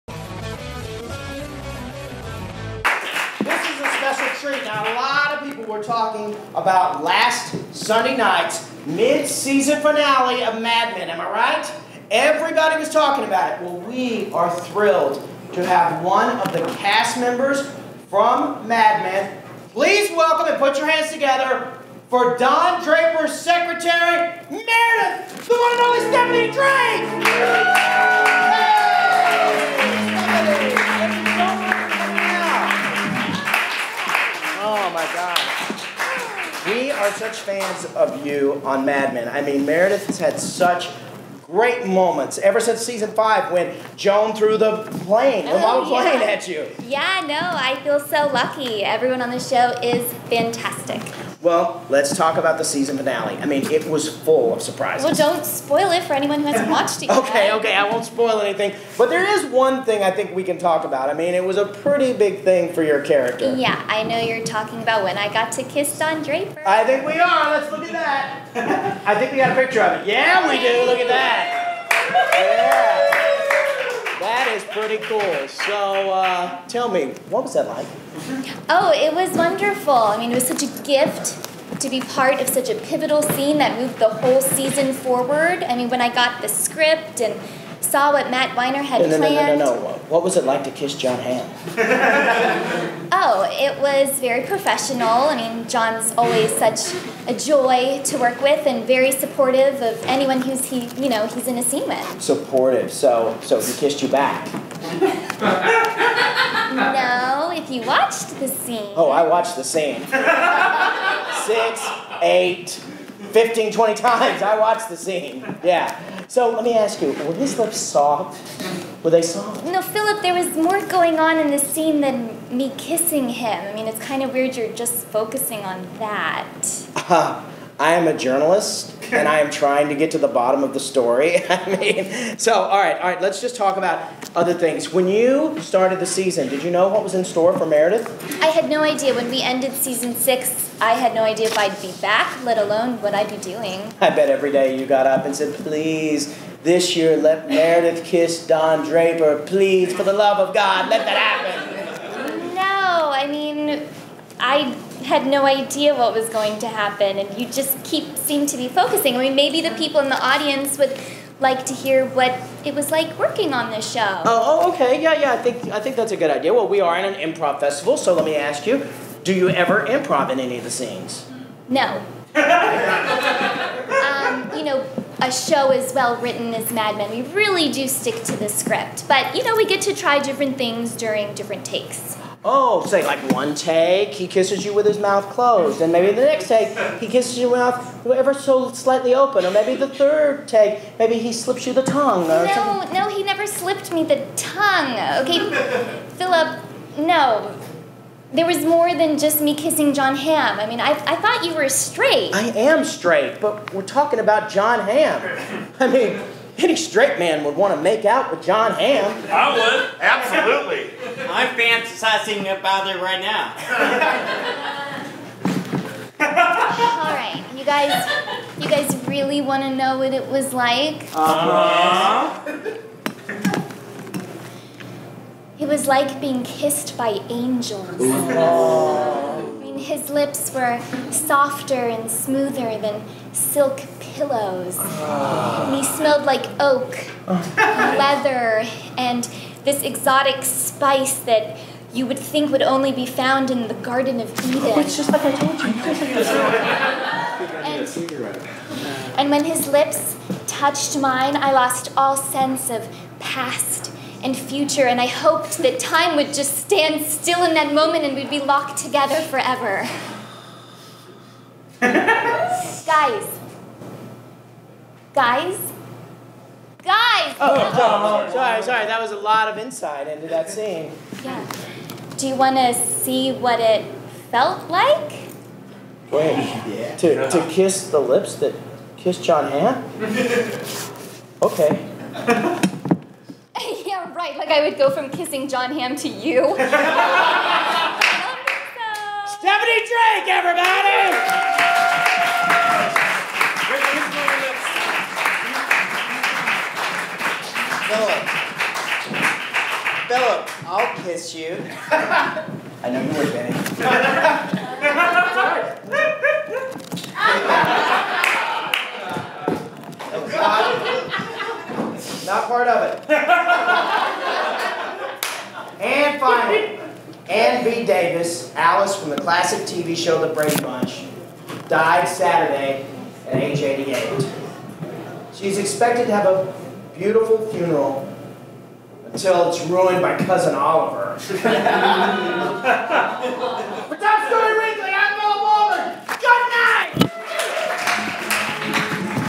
Mad Men's Stephanie Drake Talks About the "Mad Men" Season Finale
Stephanie Drake - Meredith from "Mad Men" - chats about the season finale, but all guys want to know is what it was like to kiss Jon Hamm!

Writer
Micheal Hughes

Cast
Stephanie Blake
Phillip Wilburn
Tommy Bechtold
Bennie Arthur
Josh Rachford